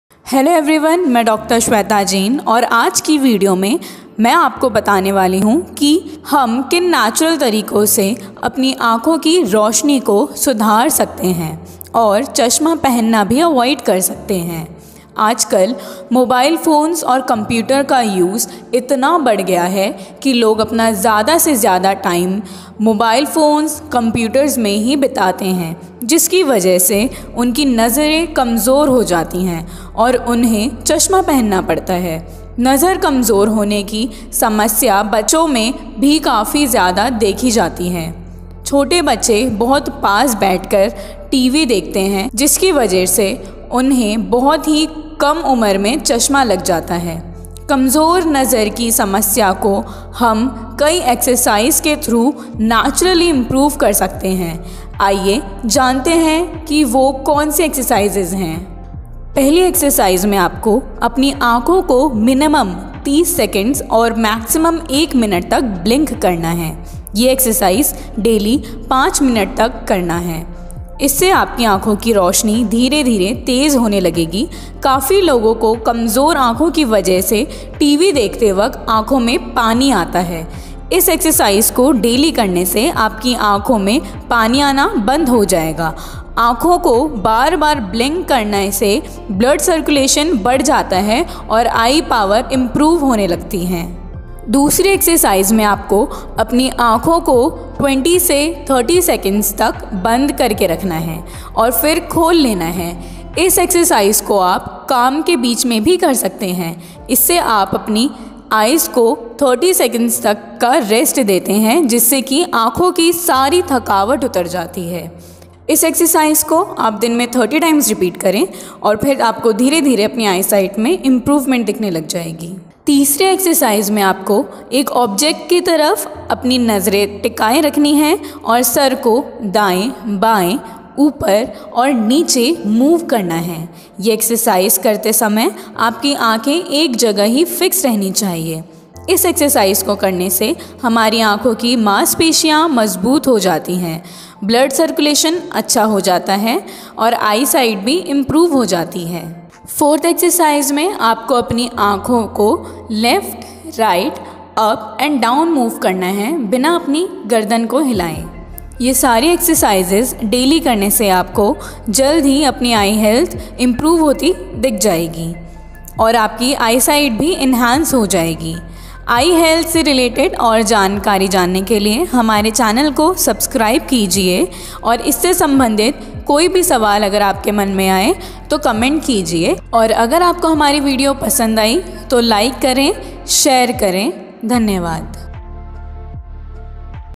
चश्मा हटाने के लिए व्यायाम | Exercises To Remove Eye Glasses
Call for any Queries: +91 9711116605

Eye problems are increasing in our day-to-day life due to the regular use of technology through gadgets. In the early stage of our life, we have started facing the problems of weak eyesight. Therefore, we need to do some eye exercises to remove the specs without surgery.

⏱ TIME-STAMPS

0:00 नेत्र व्यायाम/ Introduction
0:59 आँखों की रोशनी बढ़ाने के लिए व्यायाम/ Exercises to Improve Eyesight
2:49 आँखों का सर्वश्रेष्ठ हॉस्पिटल/ Best Eye Hospital in Delhi

Exercises To Remove Eye Glasses, चश्मा हटाने कैसे हटाए , specs removal eye exercises